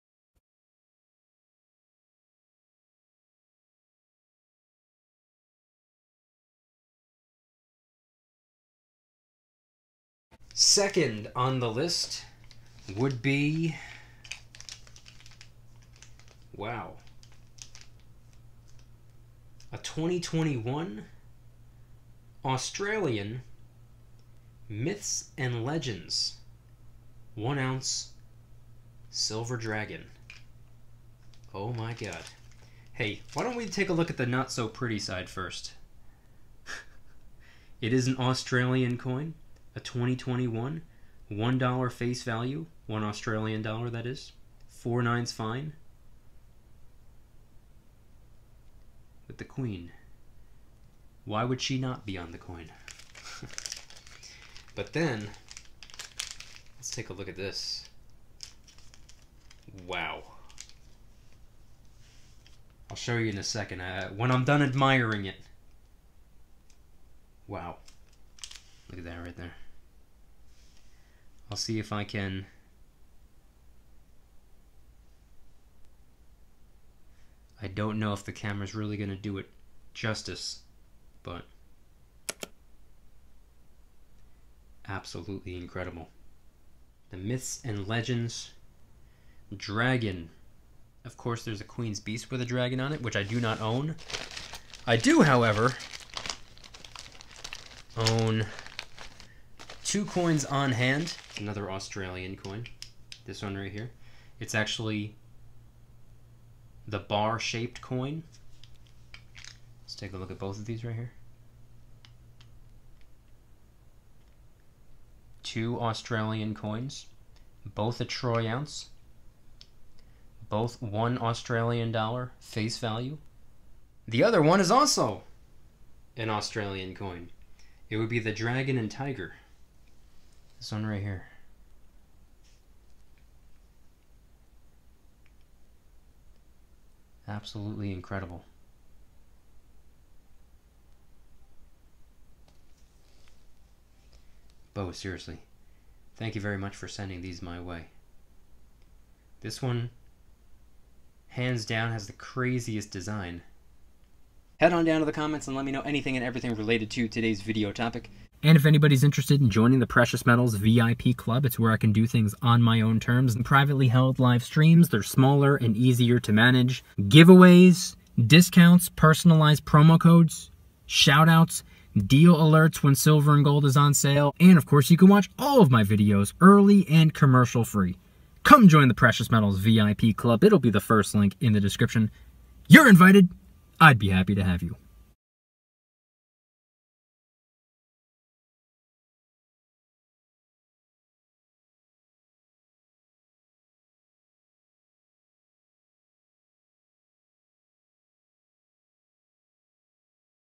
2021 1oz Australian Myths & Legends Silver Dragon Coin Unboxing & Review!
💳 Discover IT Cashback Credit Card ($50 statement credit for us both)

📚 The Compound Effect

📬 Send mail here:
Mykie DYDSS